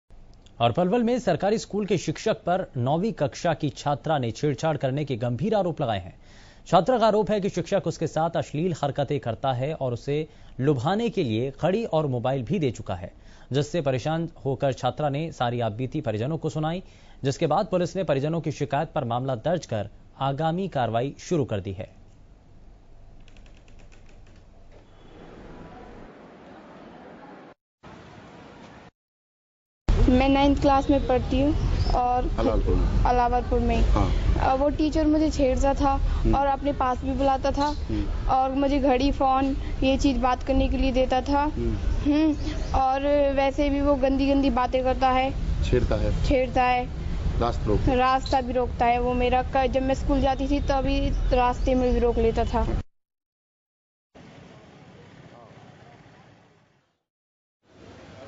शिक्षक-छात्रा का रिश्ता हुआ तार-तार, शिक्षक पर छात्रा ने लगाए छेड़छाड़ के आरोप || STV HARYANA NEWS
पलवल में सरकारी स्कूल के शिक्षक पर 9वीं कक्षा की छात्रा ने छेडछाड़ करने के गंभीर आरोप लगाए है. छात्रा का आरोप है कि शिक्षक उसके साथ अश्लील हरकतें करता है और उसे लुभाने के लिए घड़ी और मोबाइल भी दे चुका है.